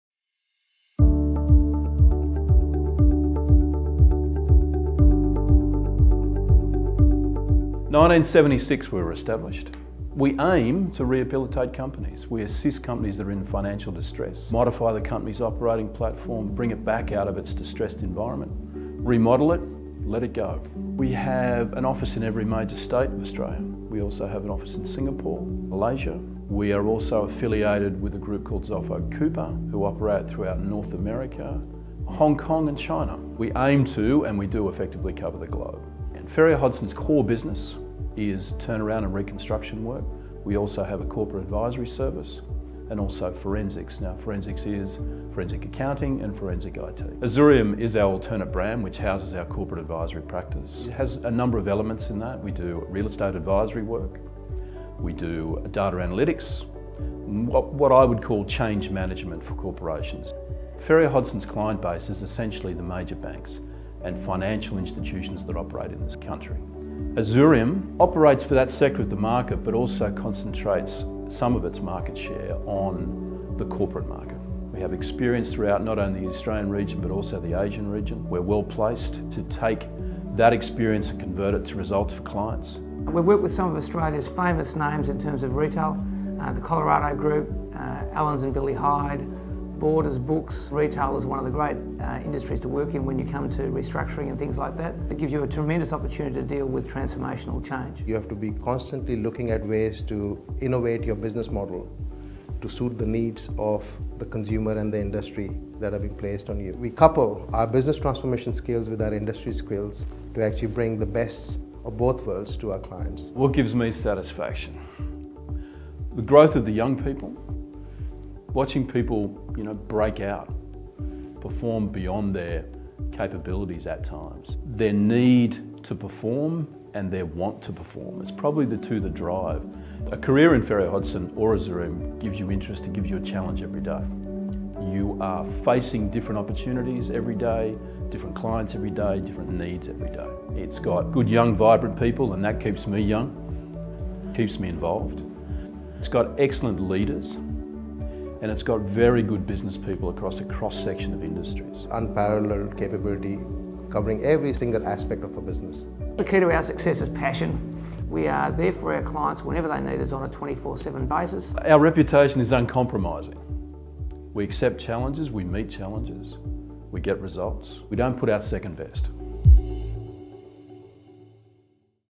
Ferrier Hodgson - Overview
Client: Ferrier Hodgson
Brief: To produce a promotional video providing an overview of the company